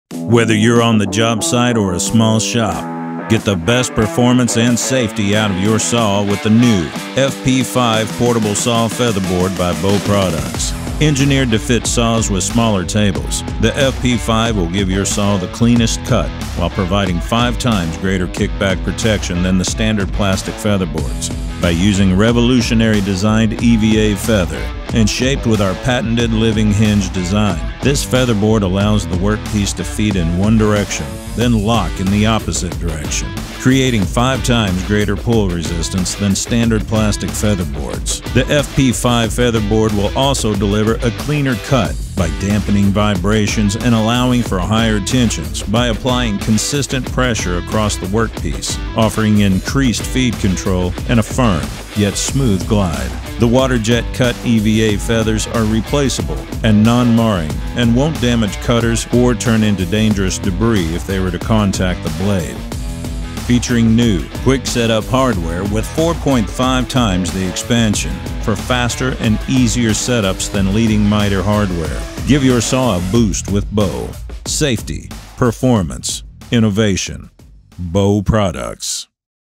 The Perfect Featherboard For Portable Saws!
The Bow FP5 Portable Saw Featherboard now brings the Safety and Performance benefits of the world’s safest featherboard “FeatherPRO” to DIY and Contractor saws with smaller tables. Featuring our revolutionary EVA feathers, the portable saw featherboard offers unmatched safety and Performance benefits.